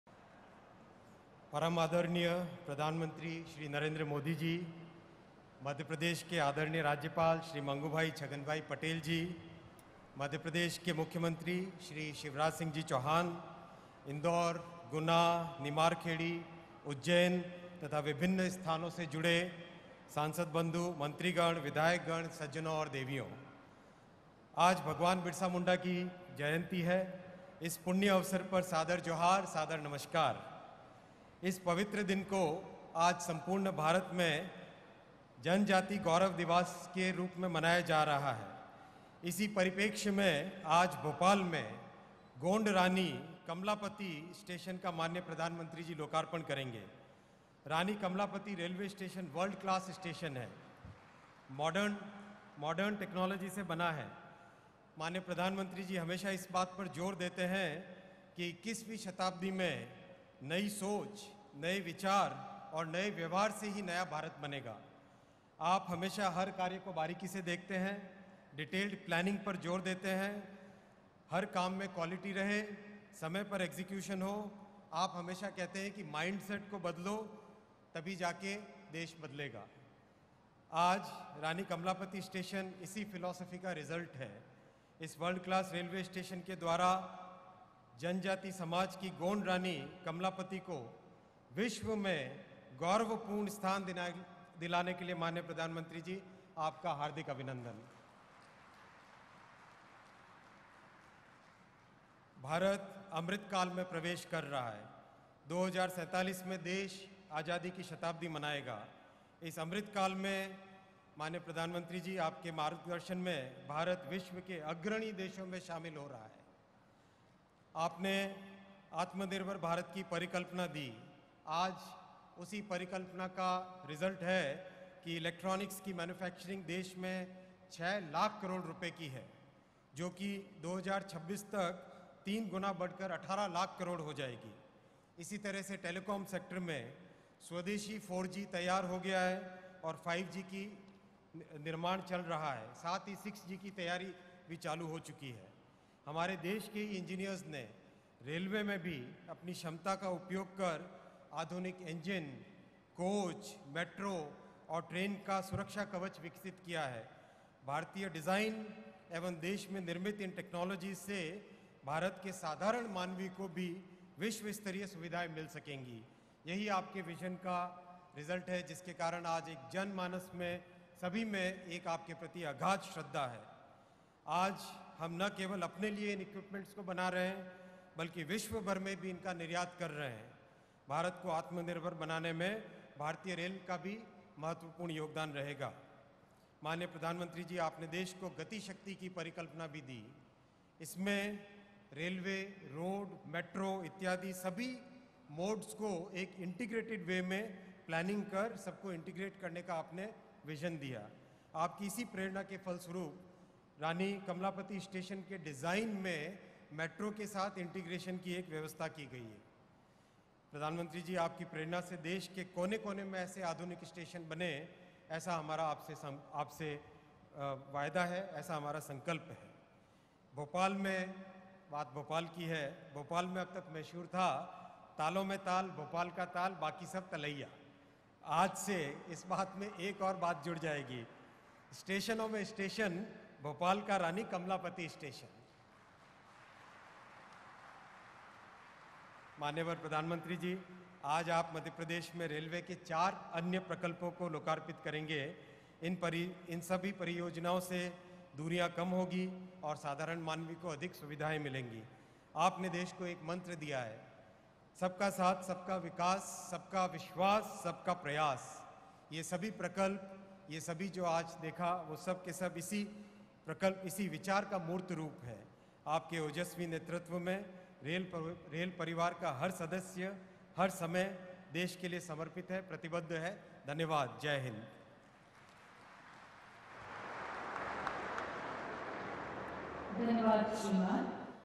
पीएम श्री नरेंद्र मोदी जी द्वारा पुनर्विकसित रानी कमलापति रेलवे स्टेशन के लोकार्पण।
पीएम श्री नरेंद्र मोदी जी द्वारा पुनर्विकसित रानी कमलापति रेलवे स्टेशन के लोकार्पण और अन्य रेल परियोजनाओं के शुभारंभ के लिए उनका हृदय से अभिनंदन।